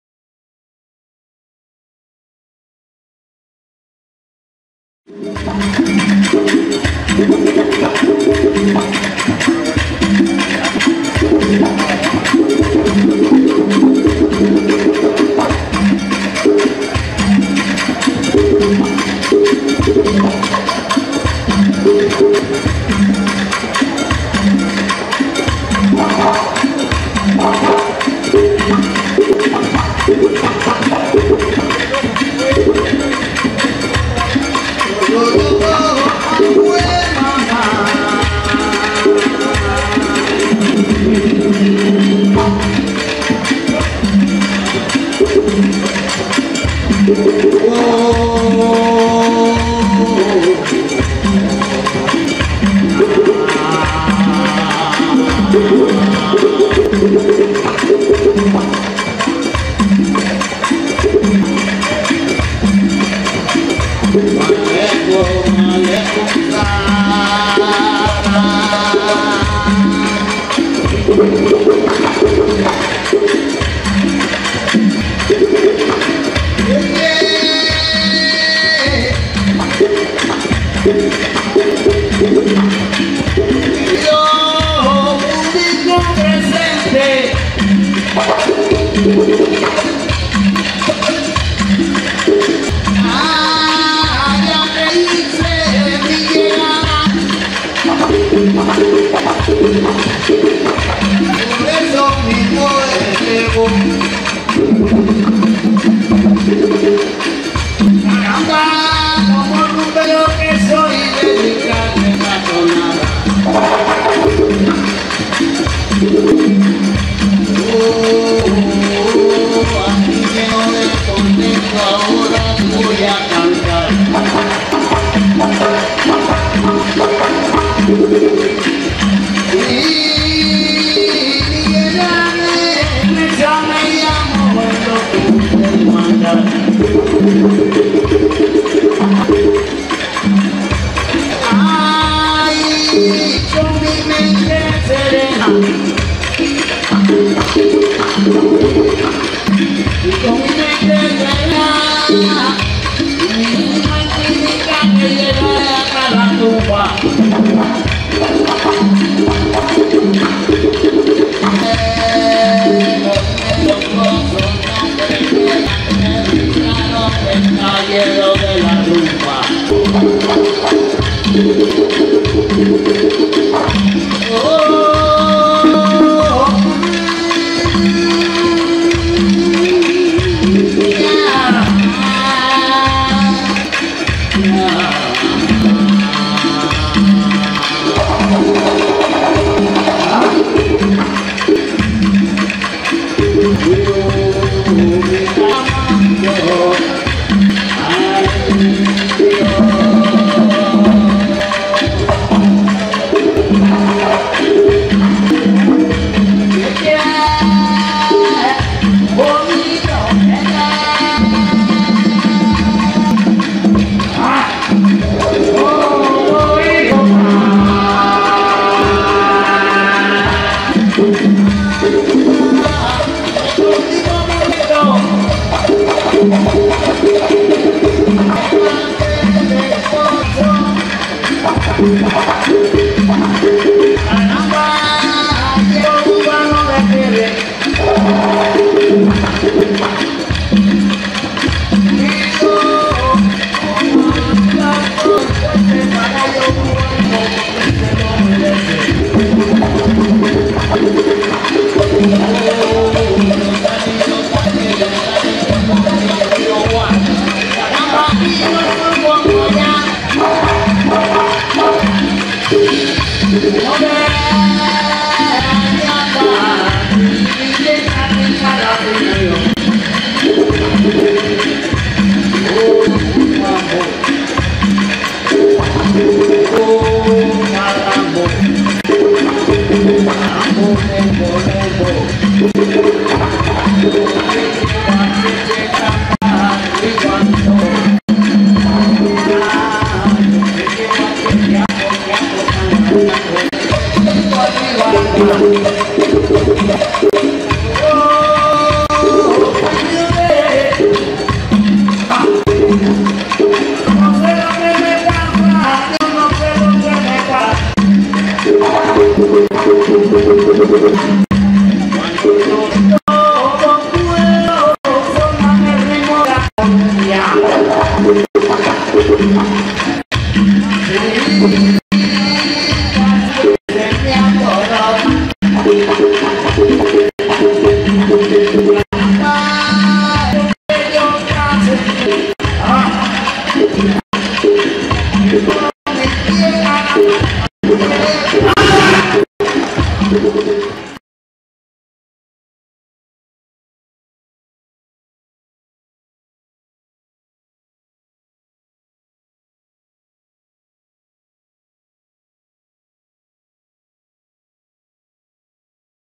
Miguel Ángel Mesa Cruz ‘Aspirina’ – canta columbia (parte I)
National Theater, Havana. October 2005, during the 20th anniversary celebration of Yoruba Andabo as an officially recognized group.